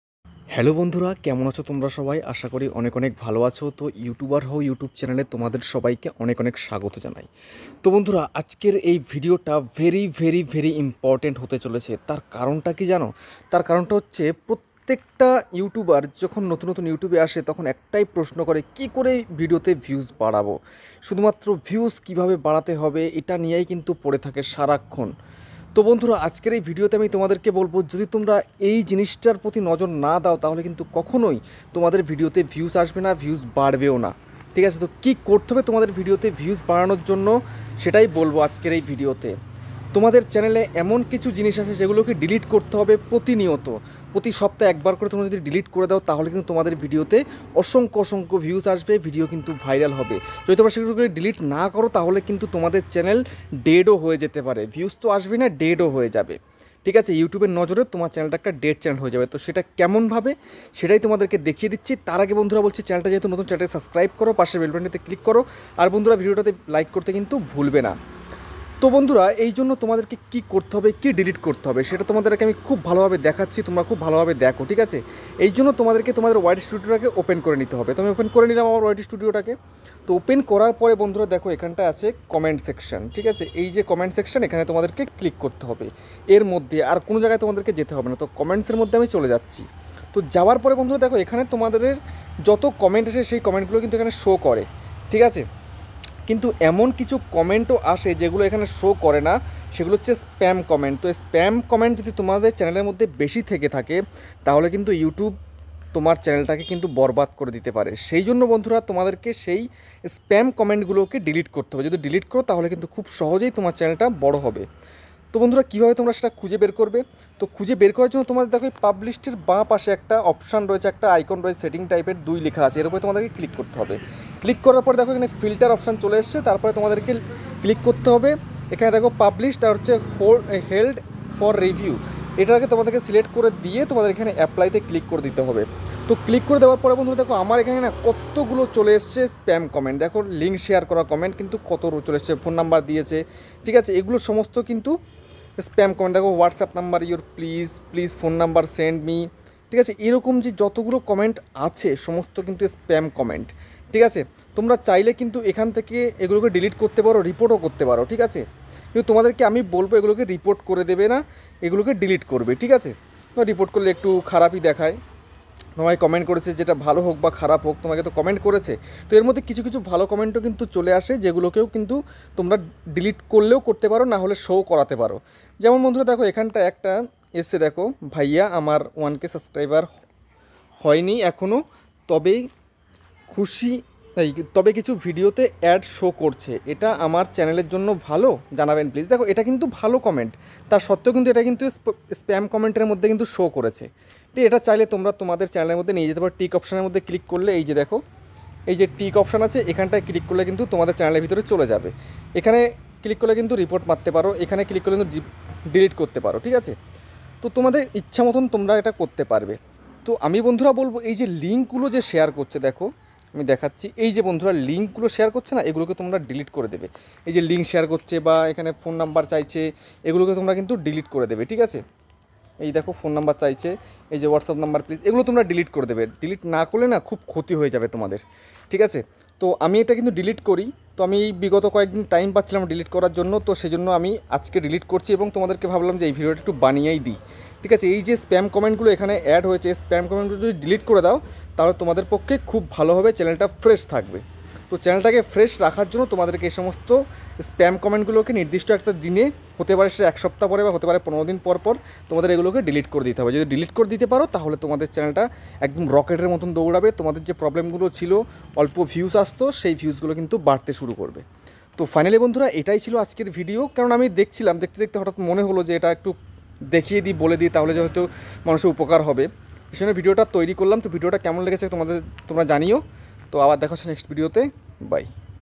চ্যানেল বাঁচাতে এগুলো এখনি ডিলিট করুন | How to protect your channel from spam comment 2023
চ্যানেল ডাউন হবে যদি মাসে ১ বার এগুলো ডিলিট না করেন | How to protect your channel from spam comment 2023